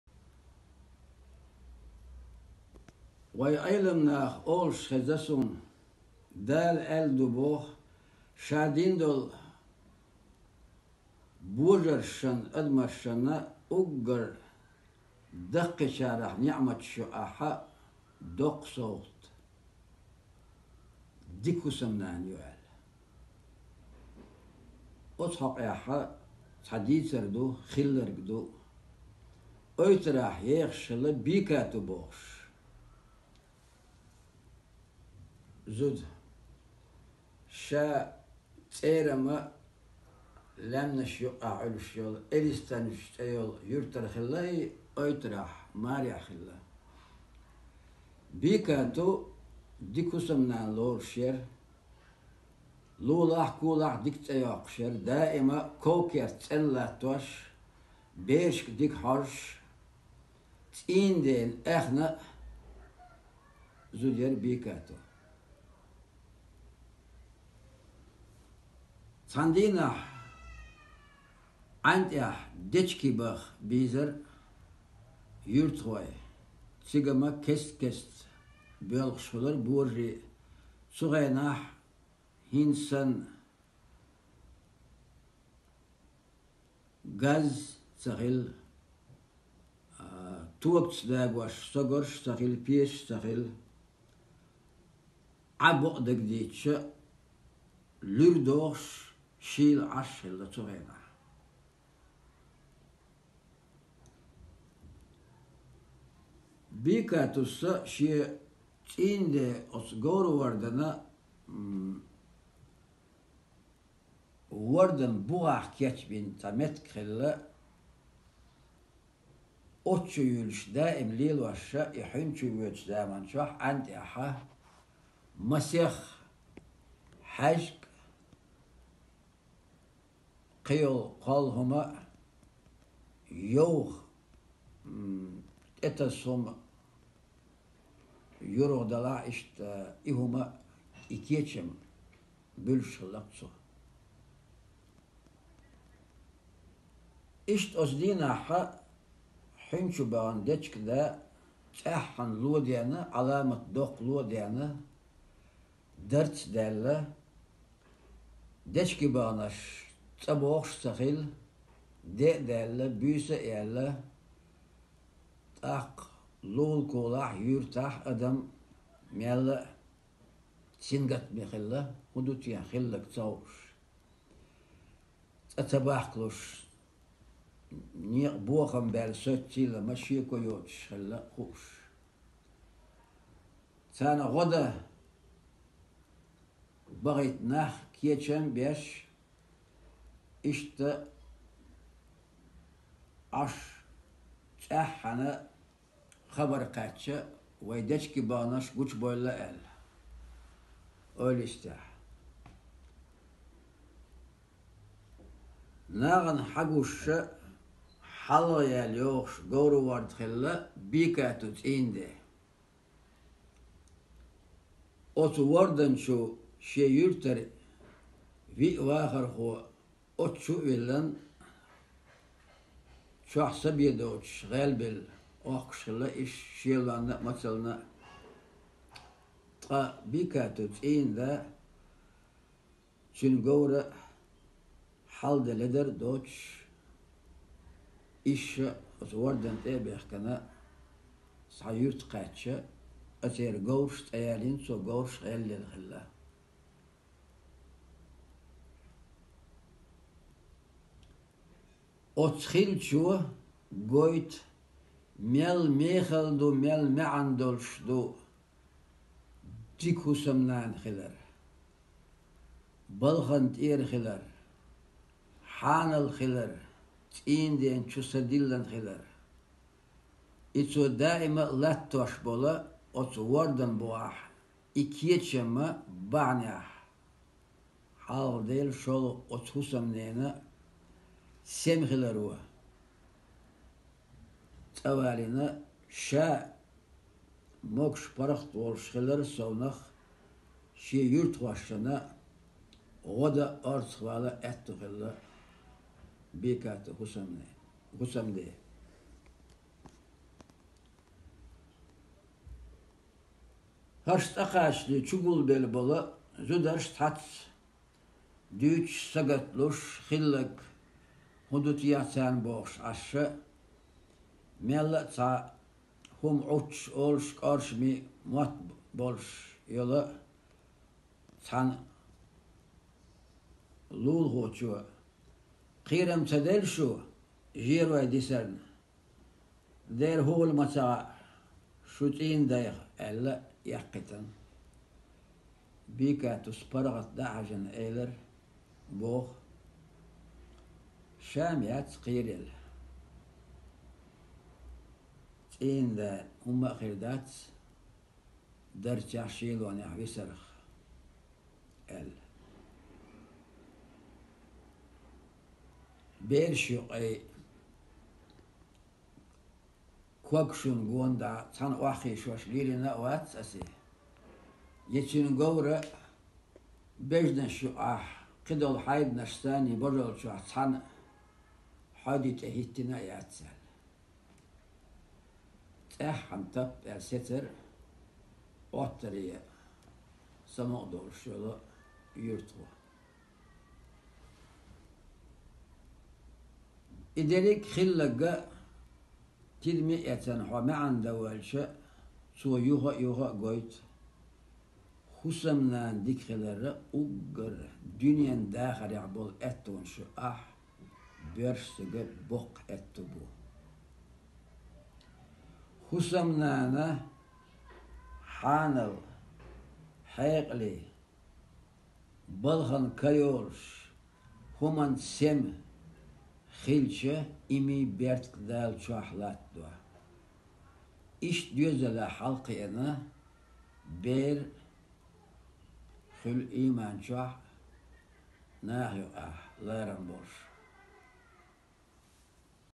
«Дика зуда - доккха беркат» «Хорошая жена – залог хорошей семьи»
Каждый народ имеет свои индивидуальные качества, свои традиции и обычаи. Они, передававшиеся из поколения в поколение, представляют собой дух и ментальность этноса. Чеченский народ не исключение. Семья в чеченском обществе, как в старину, так и сейчас играет большую роль в воспитании и формировании духовного мира подрастающего поколения.

   Конечно, в любой семье должны присутствовать уважение, любовь и поддержка. Семейные ценности чеченского народа имеют свои отличительные стороны. И любовь, и забота, уважение здесь проявляются по-своему. Жил-был один очень уважаемый человек. Однажды его спросили: «Как ты заслужил такой авторитет?» Он ответил: «Сначала я женился, и меня зауважала жена. Глядя на нее, меня стали уважать соседи, а глядя на них — все вокруг». Чеченских девушек воспитывают в соответствии с этой притчей: они должны уважать своих мужей, быть послушными и во что бы то ни стало сохранить семью «Престиж мужчины зависит от жены. Престиж семьи, тайпа - от взаимного уважения супругов